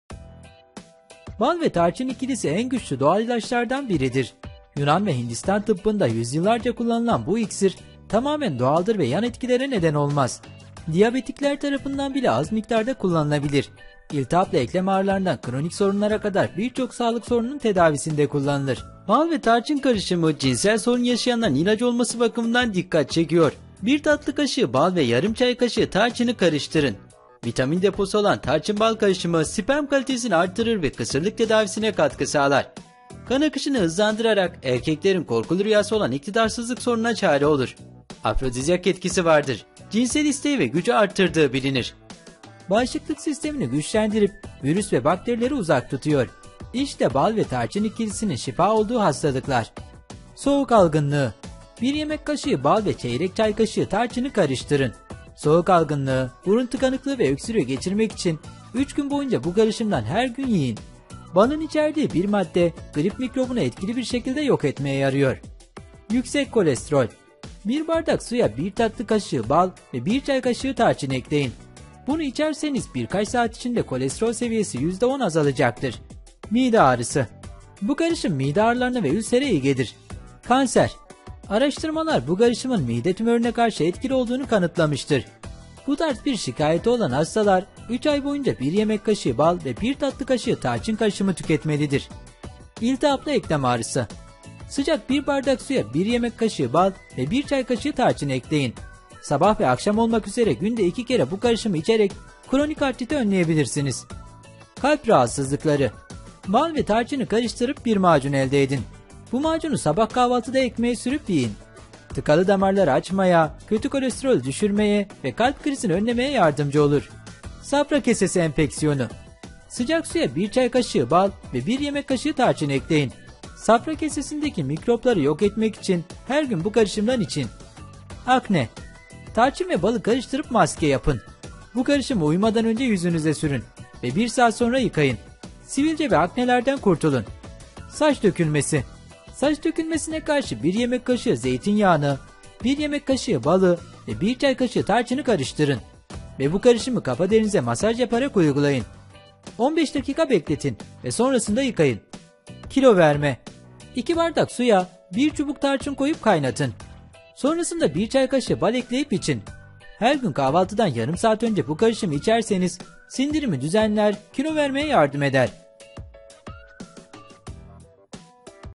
ZİRVE YAPTIRIYOR. Cinsel güç sorunu yaşayanlar için sağlıklı tarif!
Bal ve tarçın karıştırılıp tüketildiğinde bağışıklık sistemini güçlendirir, doğal bir antibiyotiktir. Birçok vitamin ve yüksek miktarda demir içeren karışım, vücudu bakteri ve virüslere karşı korur. İdrar Yolu Enfeksiyonunu tedavi ederek, İdrar yollarında oluşan mikropları öldürür.